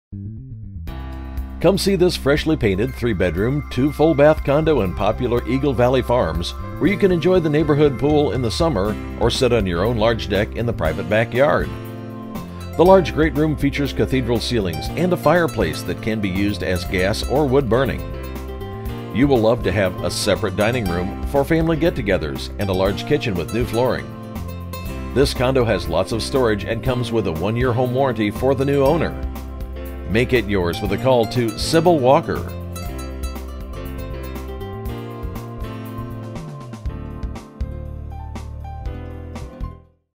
2925 Horse Hill East Drive Indianapolis, IN | MLS# 21631857
2925 Horse Hill East Drive, Indianapolis, IN

Come see this freshly painted 3 BR 2 full BA condo in popular Eagle Valley Farms, where you can enjoy the neighborhood pool in the summer or sit on your own large deck in the private backyard. You find a bedroom downstairs with a bath and walk-in closet, the large great room features cath clgs and a f